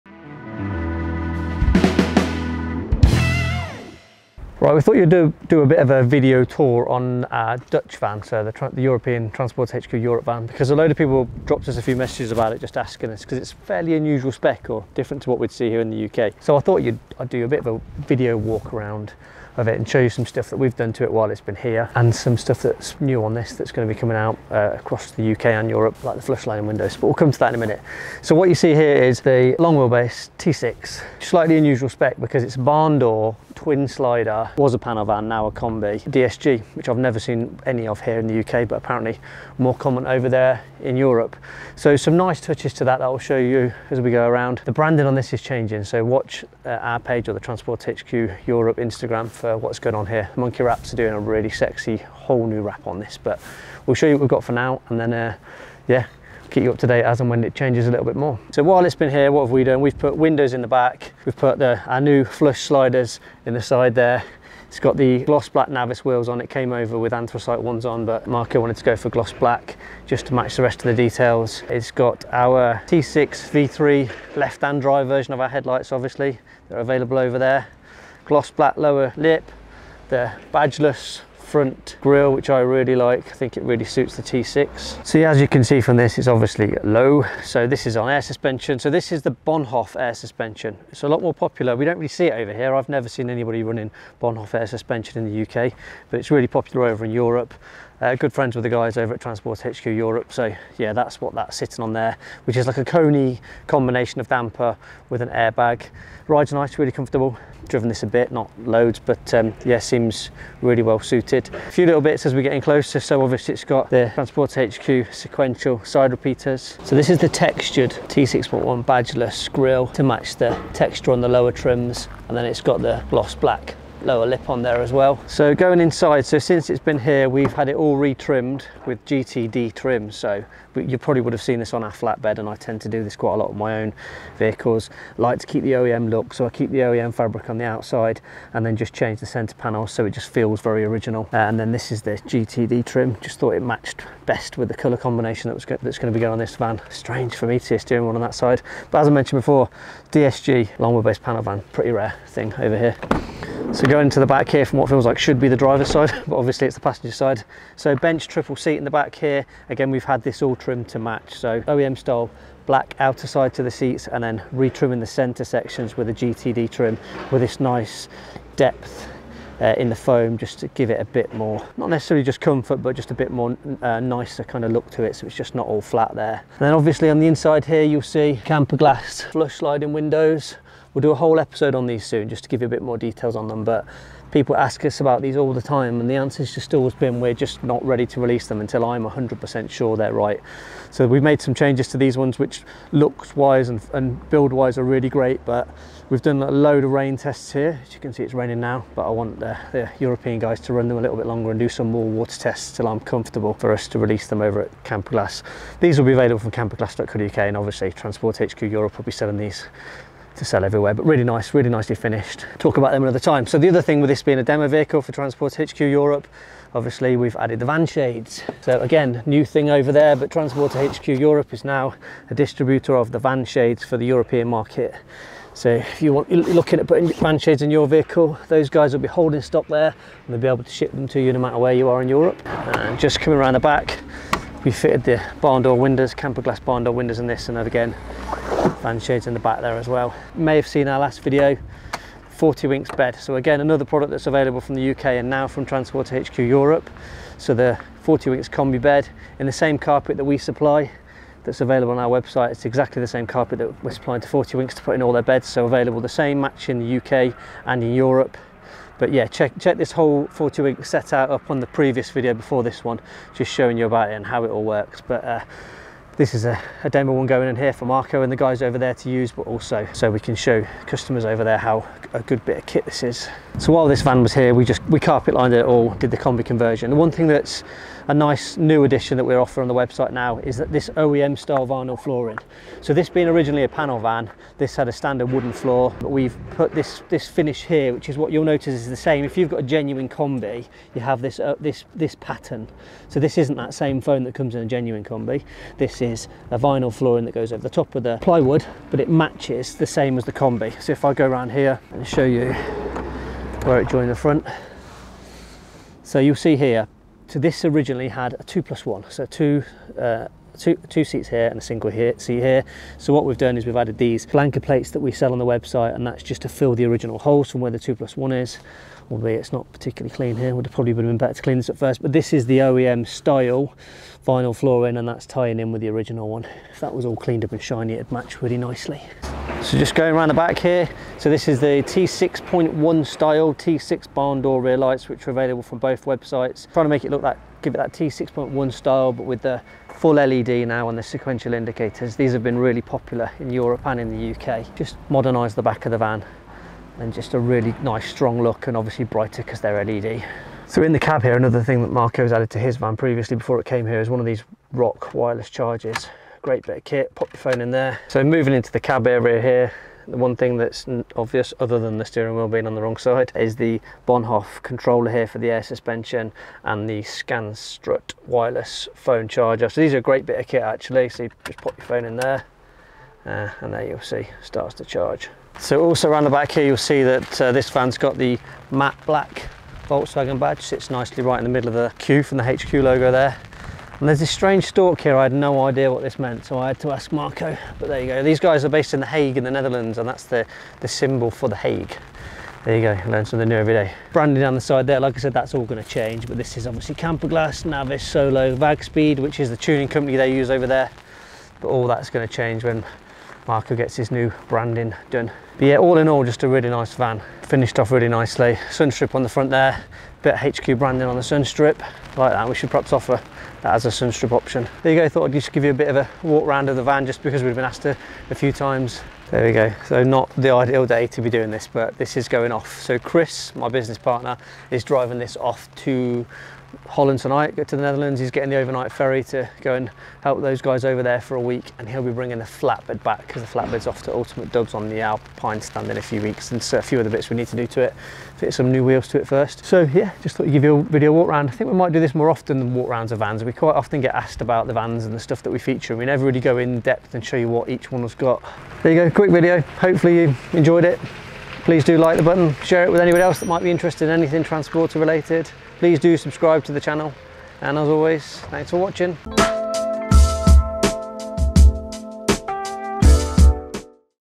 Walk round of the Transporter HQ Europe bagged LWB T6
We get asked to do van tours / video walk rounds all the time. First let's start with our Transporter HQ Europe bagged LWB T6 DSG.  There is something about long and low that just works! I love this van. It's not over the top, it's not too flashy, but still sexy, useable, practical and importantly, low.

Finally the Camper Glass flush sliding window will be here soon. I'm heading to THQ Europe in 2 weeks to film a full video and updates with dates and prices on this window. Trust me, it'll be worth the wait.